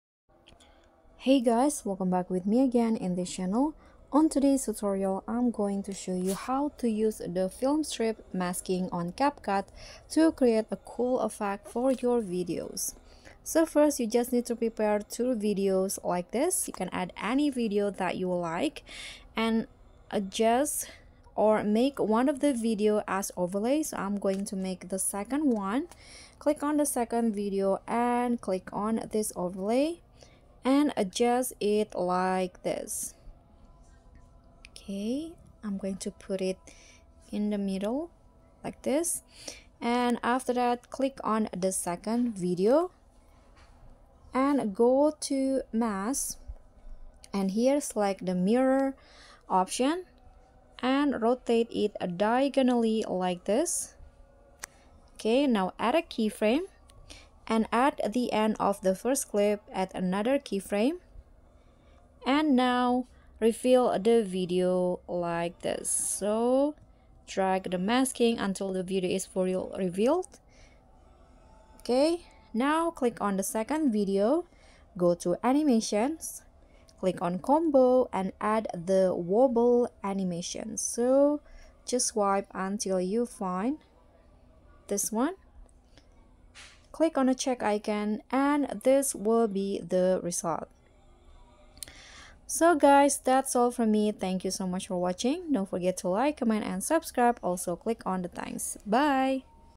[NEW UPDATE] How to Use the Film Strip Masking on CapCut to Create a Cool Effect for Your Video?
Learn how to use the film strip masking on CapCut to create an awesome video